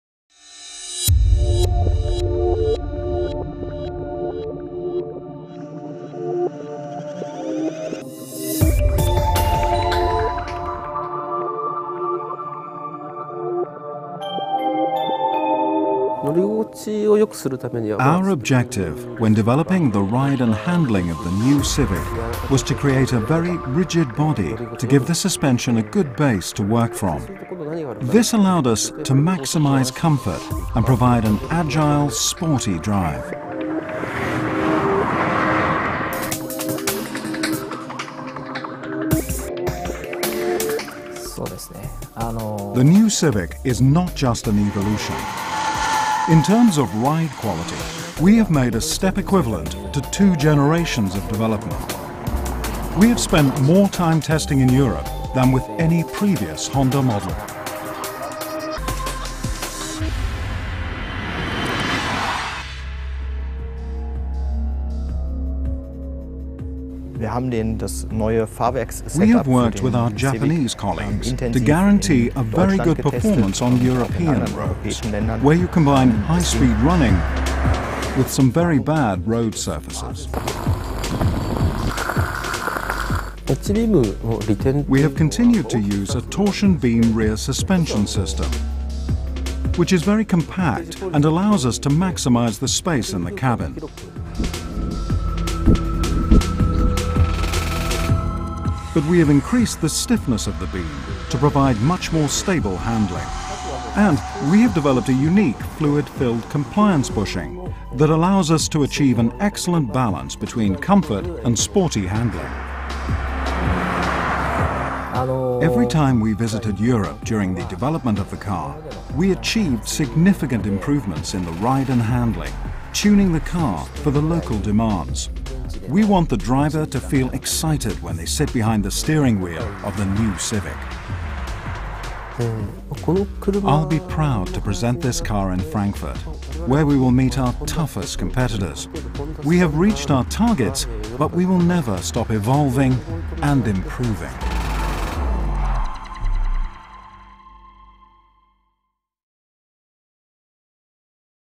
Nuova Honda Civic 2012: sviluppo telaio e handling
I tecnici di Honda Europe spiegano le evoluzioni su telaio e dinamica della Nuova Honda Civic che debutterà al Salone di Francoforte 2011